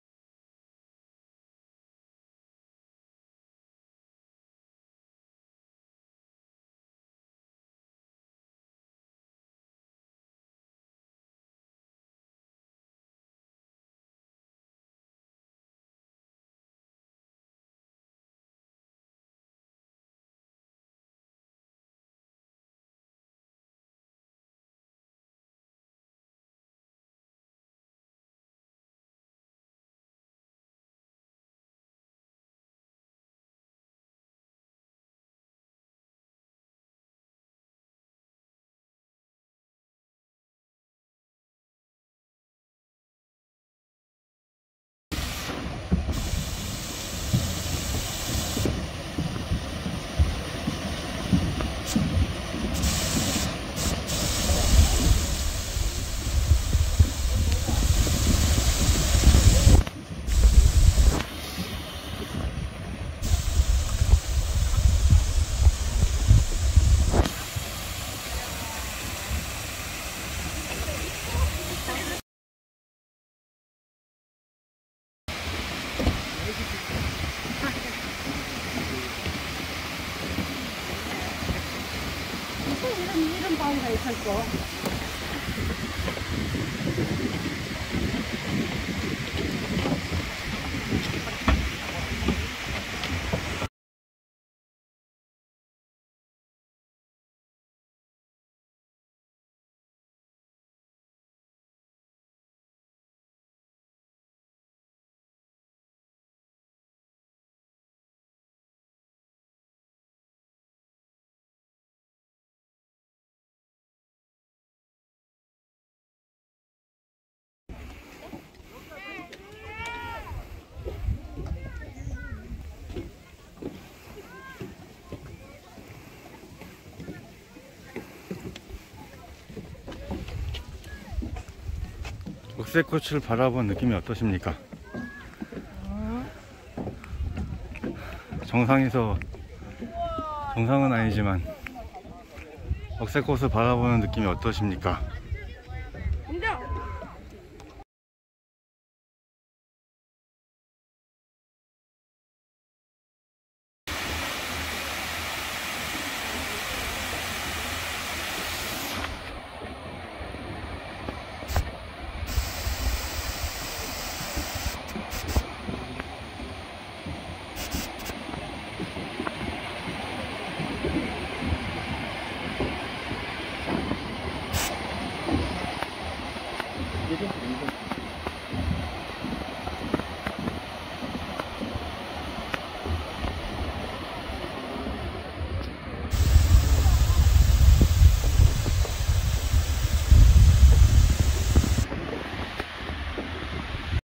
명성산 억새꽃 나드리 [ 주말 주차정보 ]
명성안 억새꽃 축제장 가는 방법 및 주차장 안내